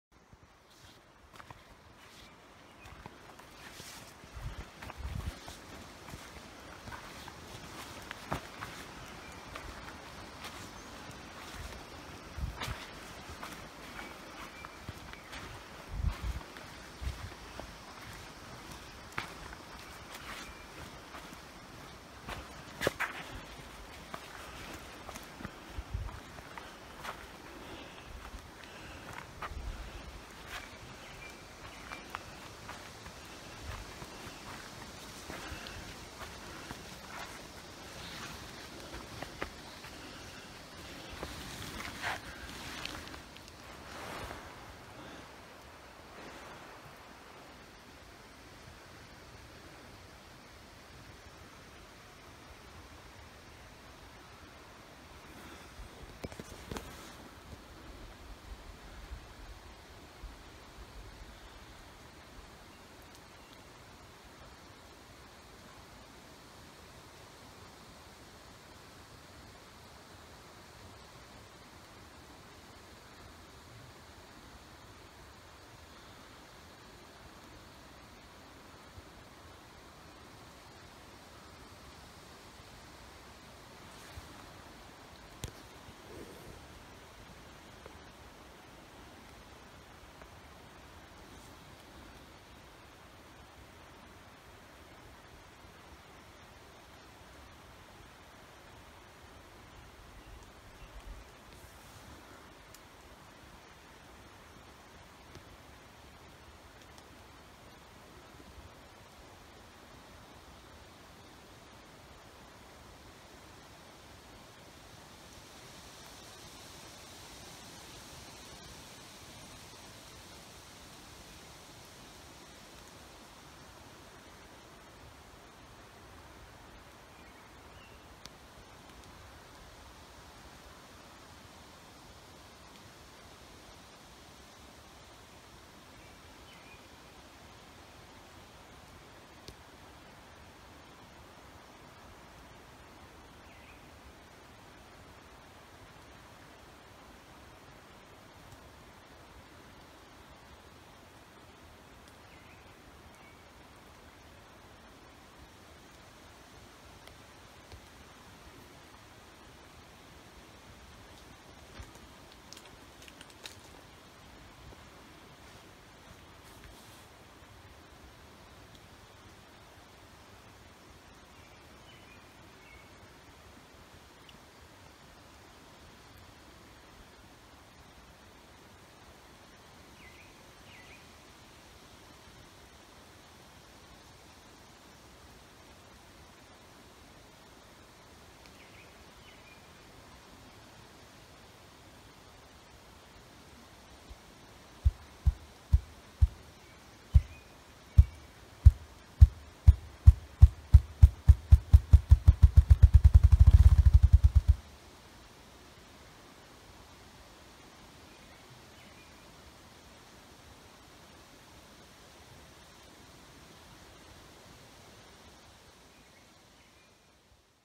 Grouse Call (South Lake, Boundary Waters Canoe Area, Cook Cty, MN May 2016)
He does his thing at 3:10.